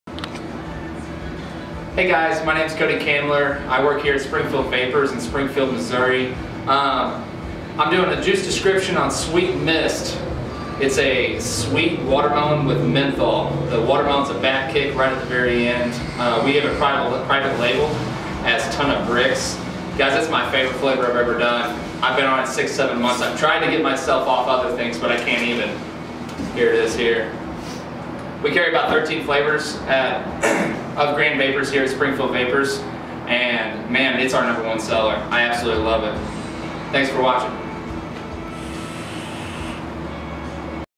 Sweet Myst (Ton of Bricks) Customer flavor description from Grandvapors.com
We have decided to let our actual customer video describe each flavor and why they like them for us. To check out this flavor and many others go